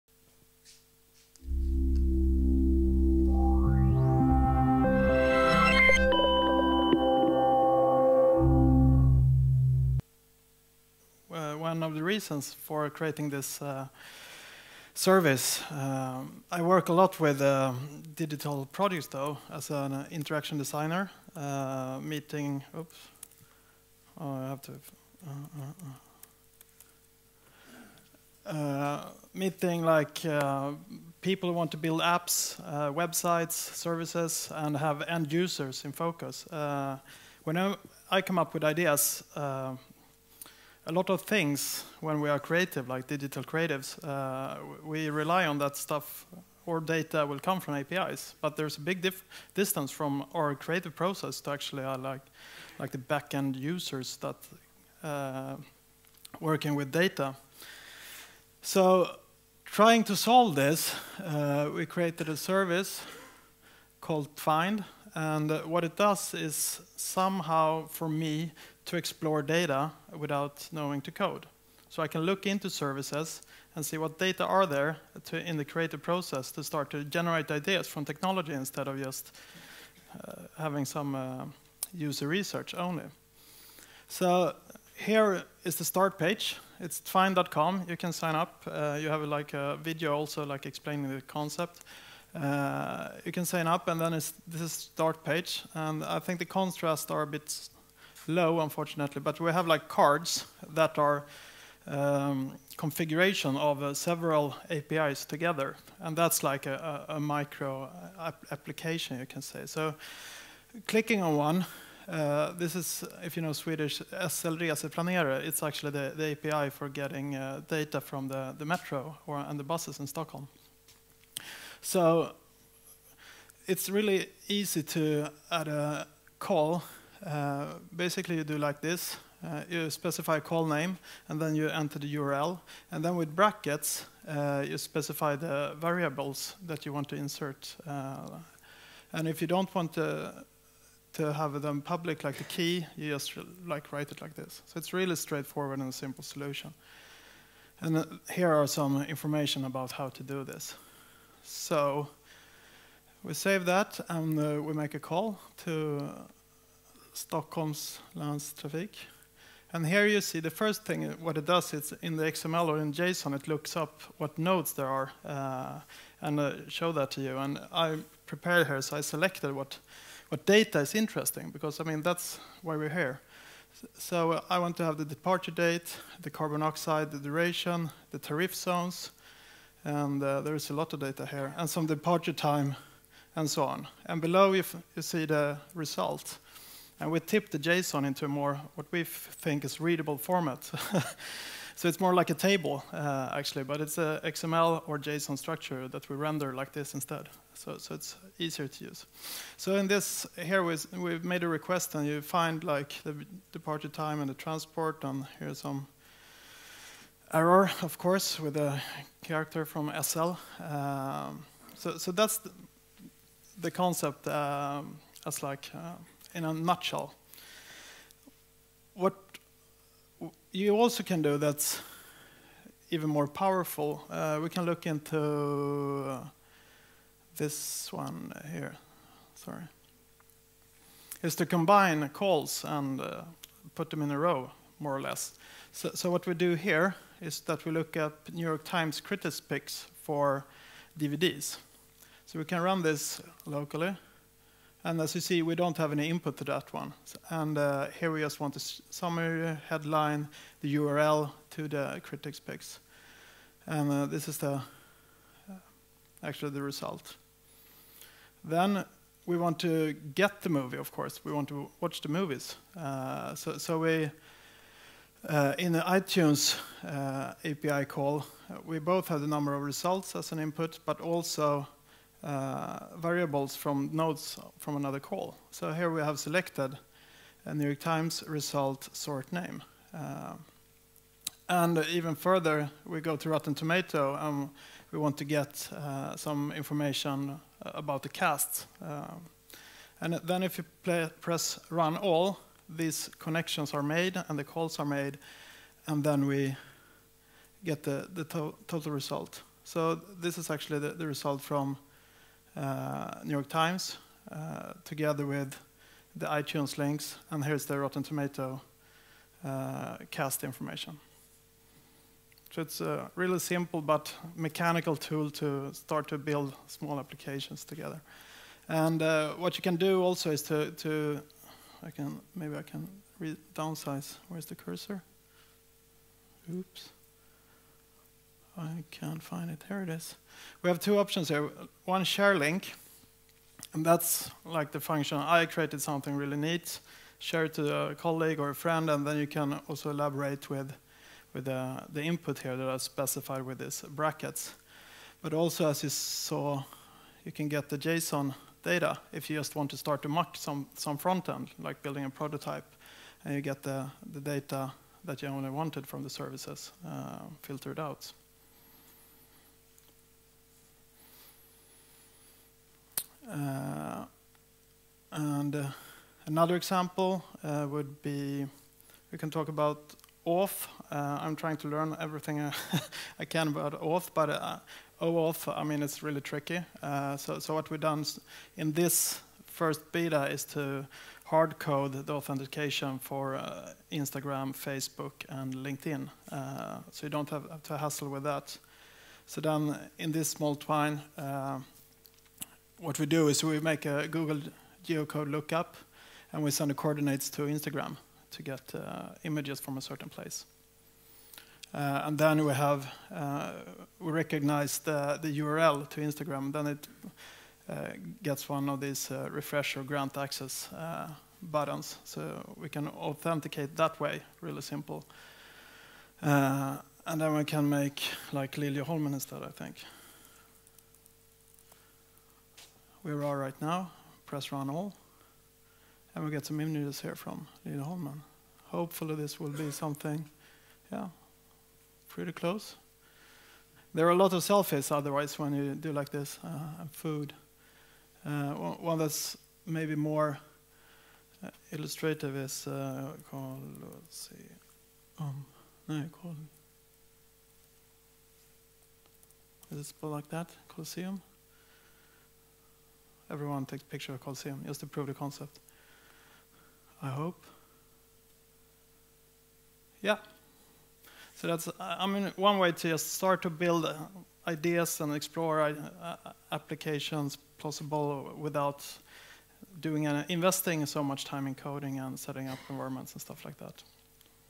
Tvined Demo
Gert Sylvest - Tradeshift - 22/10/2014
In terms of connectivity, B2B is completely uncharted land. Compared to the change that social tech has effected in other spheres, medium- and large size businesses connect as in the last millennium – point-to-point, with fenced-in data. Digital supply chains are still reserved for the worlds 0.1% largest businesses, and the the opportunity to use social tech to break down some of the digital divides have been missed. Compared to the consumer space, business is special and the invoice has a 5000 year old legacy. This all affects the way an API for B2B in the globalized world should look.

Attend this exciting presentation to learn more.

The presentation was recorded at the 2014 Platform Summit in Stockholm, Nordic APIs second annual conference.